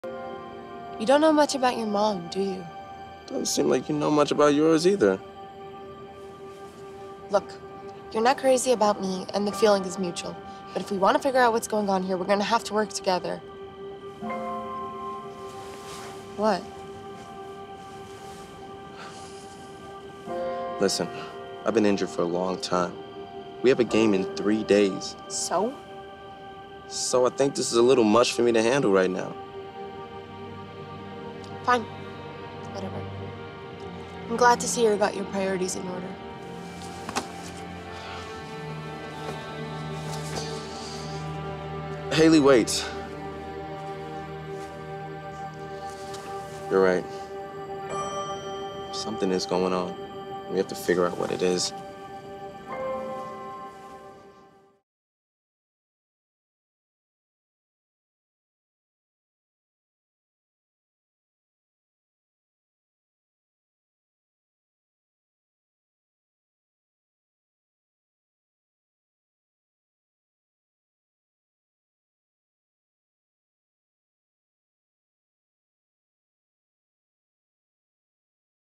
Beginning of Dayley
The first time that Daniel and Hayley decide to team up and figure out what's going on at the Greenhouse